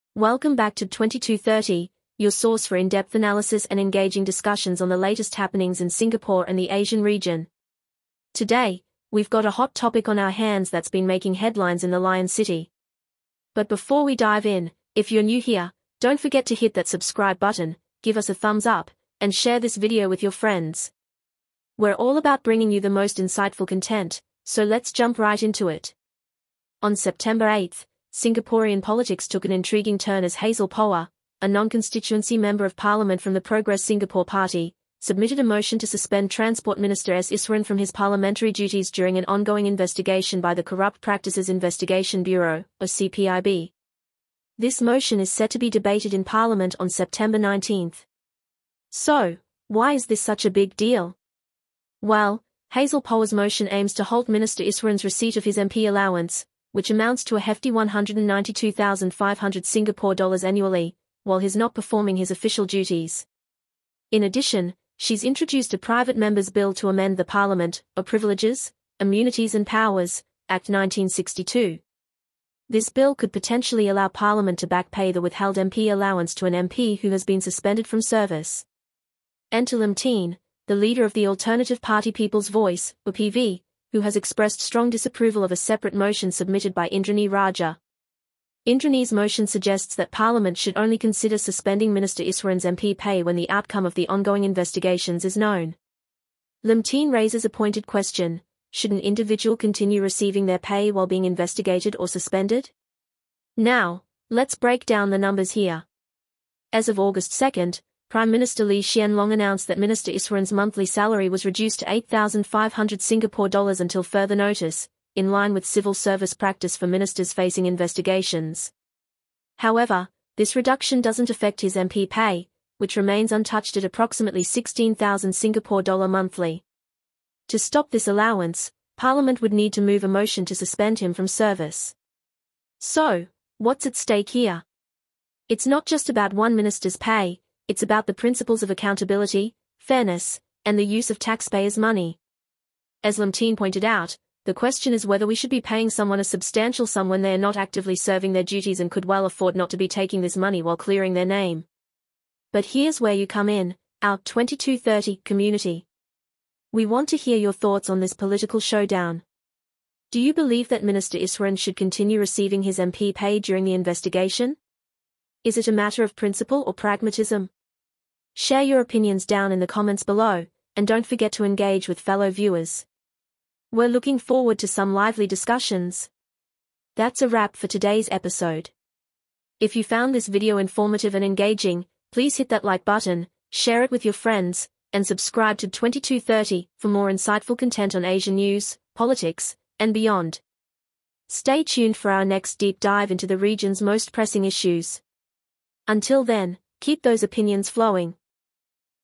Singapore's Political Showdown: Lim Tean - Is Suspension of Minister S Iswaran Justified?
On September 8, Singaporean politics took an intriguing turn as Hazel Poa, a Non-Constituency Member of Parliament from the Progress Singapore Party, submitted a motion to suspend Transport Minister S Iswaran from his parliamentary duties during an ongoing investigation by the Corrupt Practices Investigation Bureau (CPIB). This motion is set to be debated in Parliament on September 19.

So, why is this such a big deal? Well, Hazel Poa's motion aims to halt Minister Iswaran's receipt of his MP allowance, which amounts to a hefty S$192,500 annually, while he's not performing his official duties. In addition, she's introduced a Private Member’s Bill to amend the Parliament (Privileges, Immunities and Powers) Act 1962. This bill could potentially allow Parliament to back-pay the withheld MP allowance to an MP who has been suspended from service.

Enter Lim Tean, the leader of the alternative party Peoples Voice (PV), who has expressed strong disapproval of a separate motion submitted by Indranee Rajah. Indranee's motion suggests that Parliament should only consider suspending Minister Iswaran's MP pay when the outcome of the ongoing investigations is known. Lim Tean raises a pointed question: Should an individual continue receiving their pay while being investigated or suspended?

Now, let's break down the numbers here. As of August 2, Prime Minister Lee Hsien Loong announced that Minister Iswaran's monthly salary was reduced to S$8,500 until further notice, in line with civil service practice for ministers facing investigations. However, this reduction doesn't affect his MP pay, which remains untouched at approximately S$16,000 monthly. To stop this allowance, Parliament would need to move a motion to suspend him from service.

So, what's at stake here? It's not just about one minister's pay; it's about the principles of accountability, fairness, and the use of taxpayers' money. As Lim Tean pointed out, the question is whether we should be paying someone a substantial sum when they're not actively serving their duties and could well afford not to be taking this money while clearing their name.